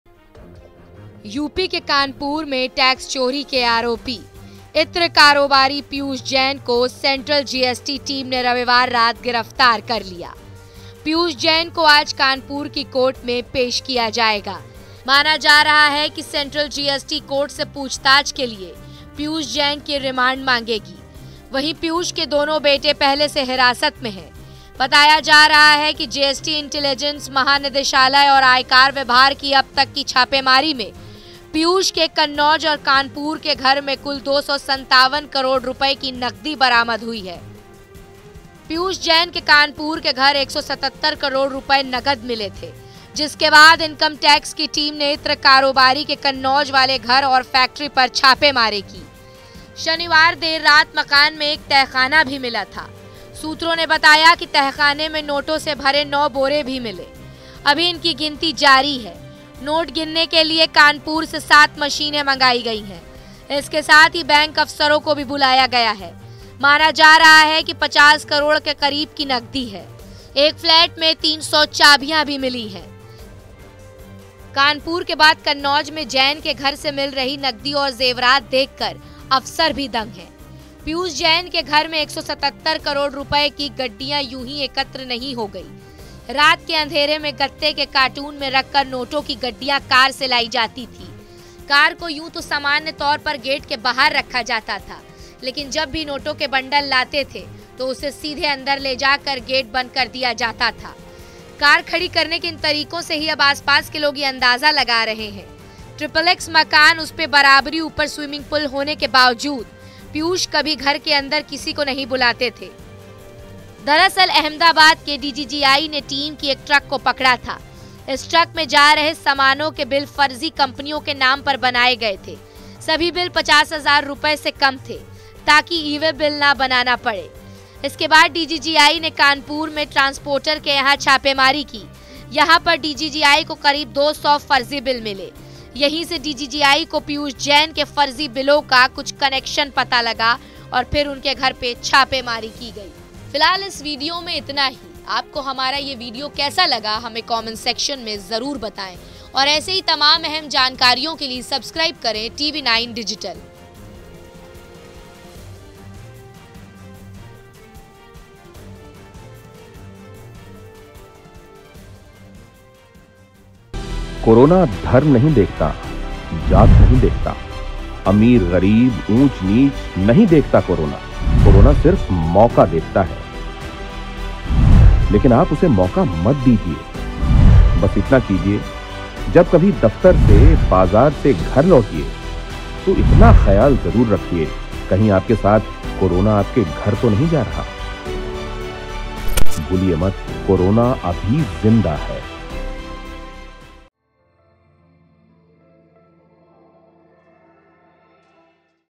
Kanpur : इत्र कारोबारी Piyush Jain गिरफ्तार, छापेमारी में अब तक 257 करोड़ कैश और जूलरी बरामद
यूपी के कानपुर में टैक्स चोरी के आरोपी इत्र कारोबारी पीयूष जैन को सेंट्रल GST टीम ने रविवार रात गिरफ्तार कर लिया. पीयूष जैन को आज कानपुर की कोर्ट में पेश किया जाएगा. माना जा रहा है कि सेंट्रल जीएसटी कोर्ट से पूछताछ के लिए पीयूष जैन की रिमांड मांगेगी. वहीं, पीयूष के दोनों बेटे पहले से ही हिरासत में हैं. बताया जा रहा है कि GST इंटेलिजेंस महानिदेशालय और आयकर विभाग की अब तक की छापेमारी में पीयूष के कन्नौज और कानपुर के घर से कुल 257 करोड़ रुपए की नकदी बरामद हुई है.

Credit : National | Abhishek/Producer | ../Editor